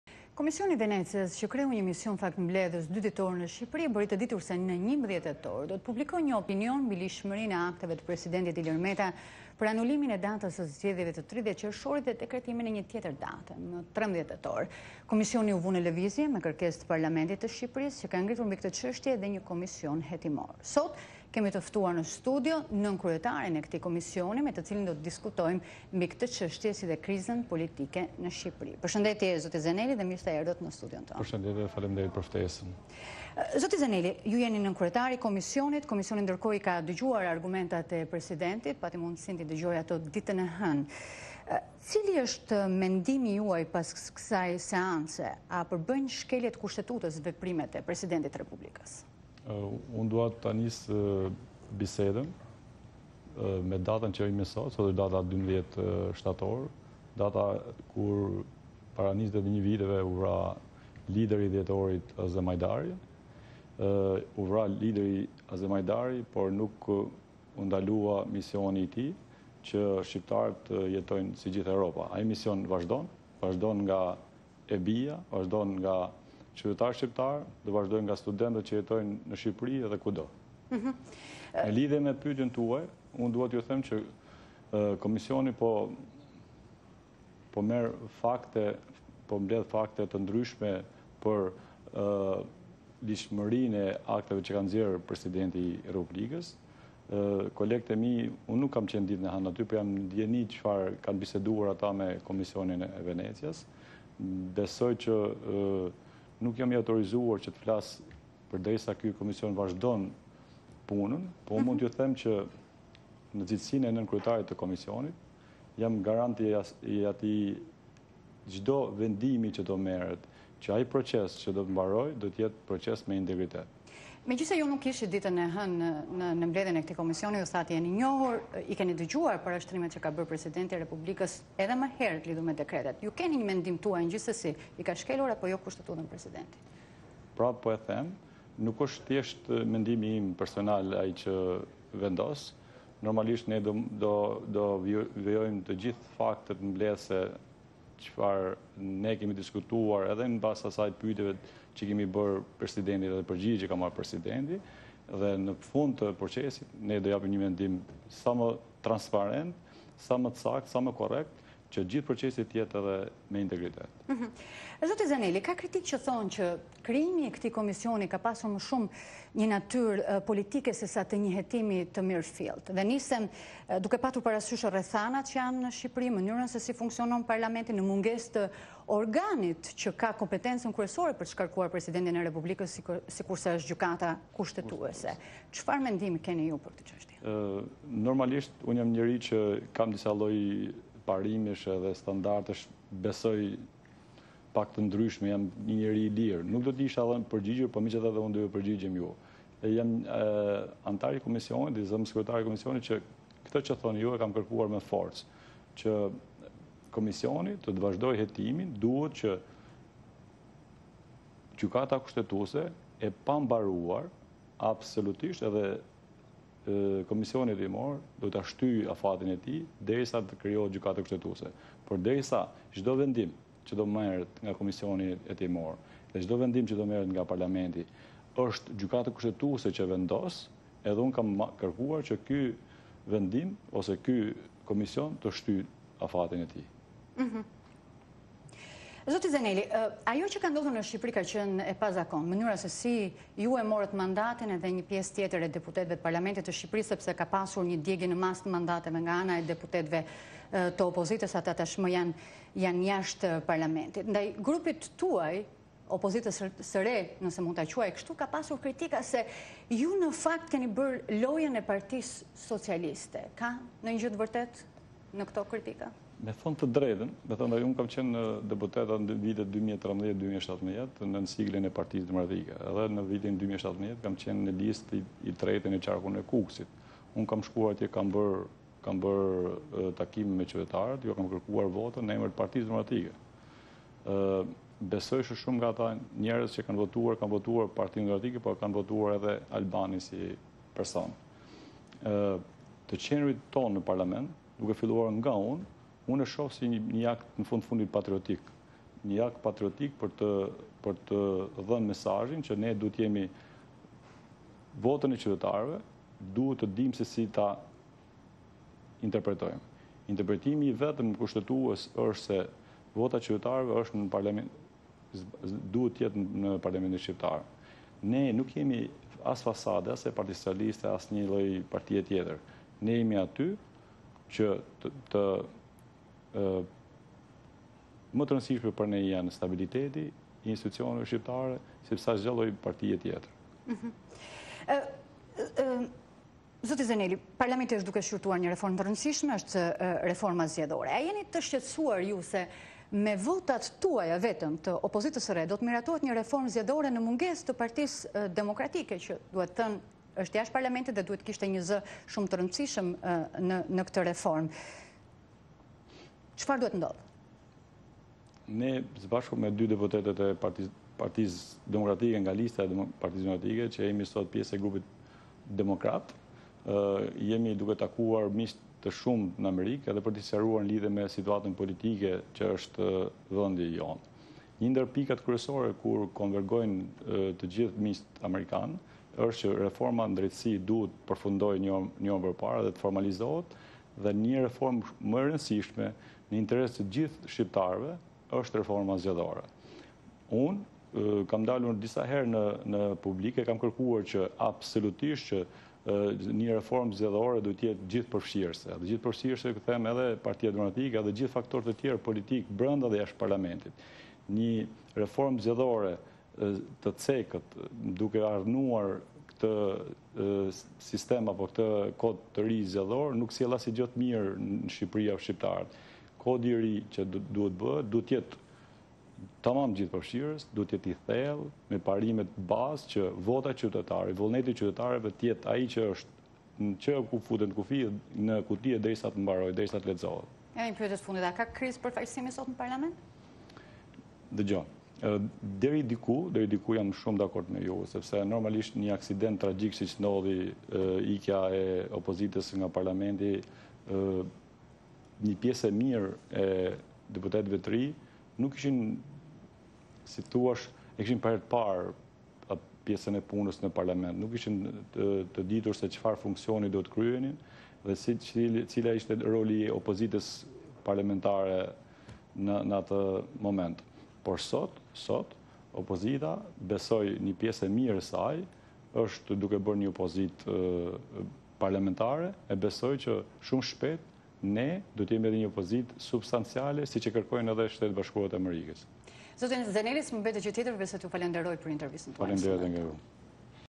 Intervistë me deputetin Alban Zeneli
Nënkryetari i komisionit hetimor që po shqyrton kushtetueshmërinë e veprimeve të ndërmarra nga Presidenti Ilir Meta për datën e zgjedhjeve foli për nevojën e ngritjes së Gjykatës Kushtetuese dhe probleme të tjera në sistemin e drejtësisë